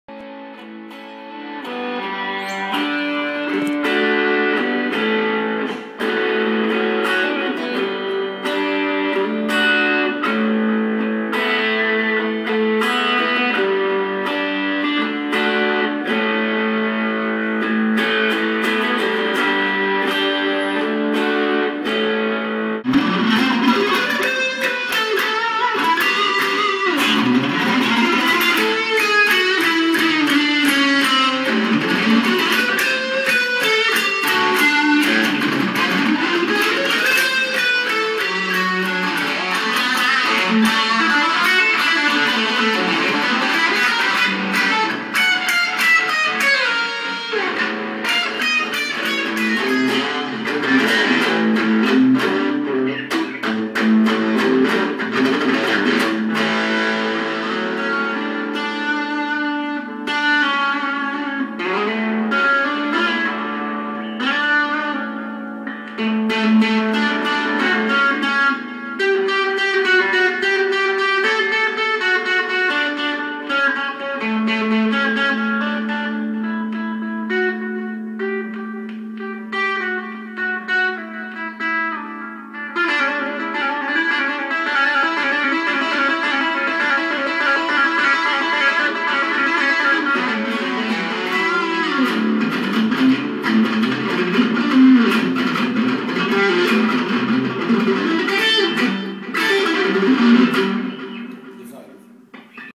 Chillin' out with Aron Walsh
Guitar just arrived today in Milano, Italy from my friend Nick Perri, chill in' out with my friend Aron Walsh in the garage.